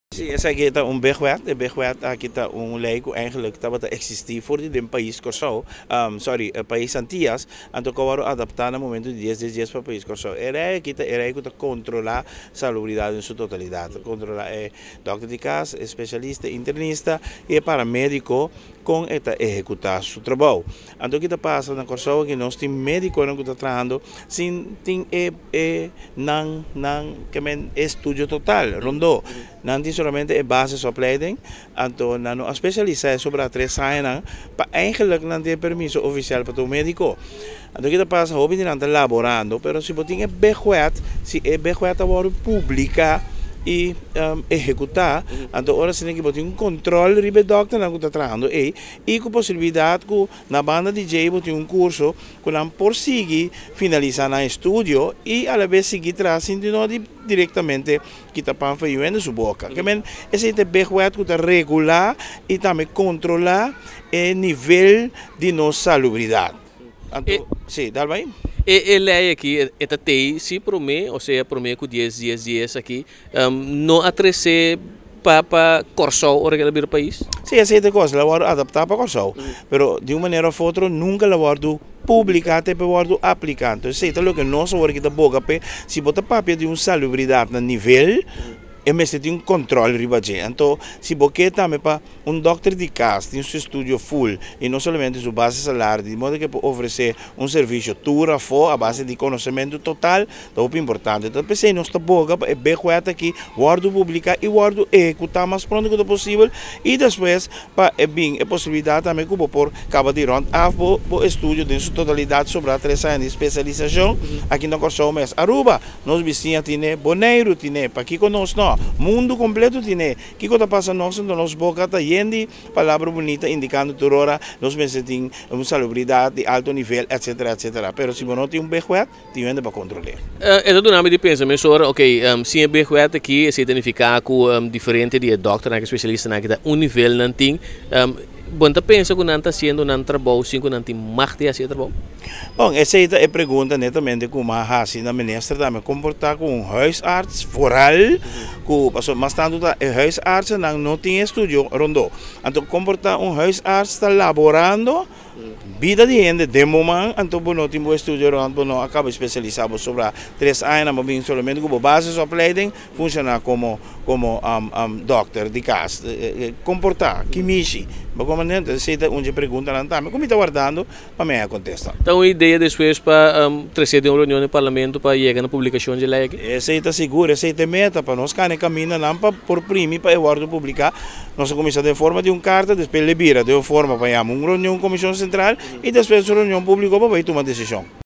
Ta insólito kon nos ta pèrmití pa dòkternan di kas profilá komo tal
Ta insólito kon nos ta pèrmití pa dòkternan di kas profilá komo tal, sin a terminá nan spesialisashon, pues tres aña èkstra despues di estudio di medisina general. Asina parlamentario di MFK Gilbert Doran ta
ekspresa den un karta ku ela dirigi na minister di Salubridat publiko Dorothy Pieters Janga. SegunDoran, aparentemente no tin un lei ku ta definí kalidat di personalnan di médiko ni paramédiko na Korsou.